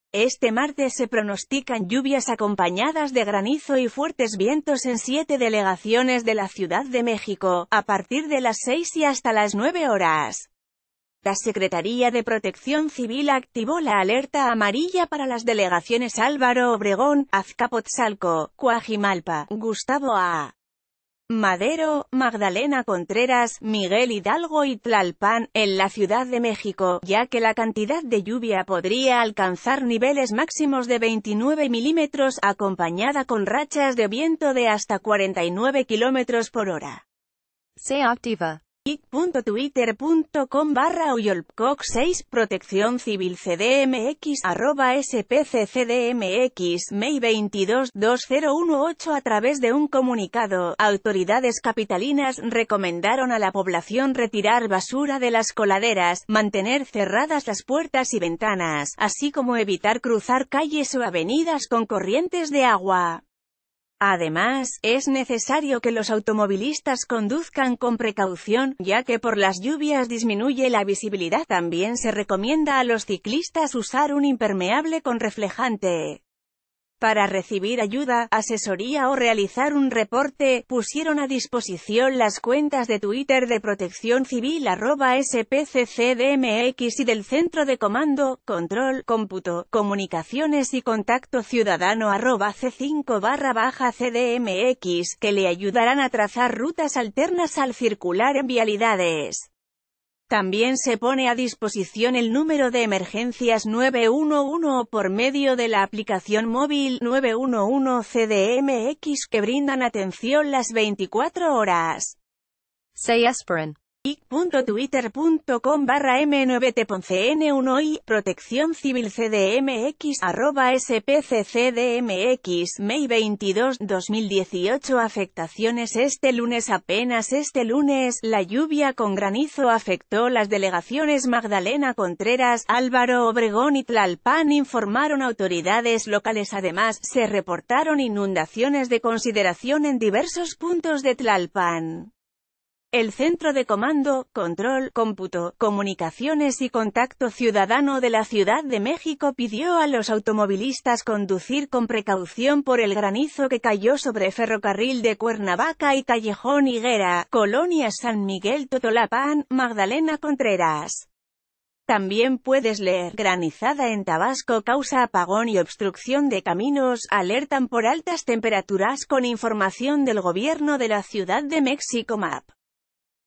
Activan Alerta Amarilla por lluvias en 7 delegaciones de la CDMX
Este martes se pronostican lluvias acompañadas de granizo y fuertes vientos en siete delegaciones de la Ciudad de México, a partir de las 18:00 y hasta las 21:00 horas. La Secretaría de Protección Civil activó la Alerta Amarilla para las delegaciones Álvaro Obregón, Azcapotzalco, Cuajimalpa, Gustavo...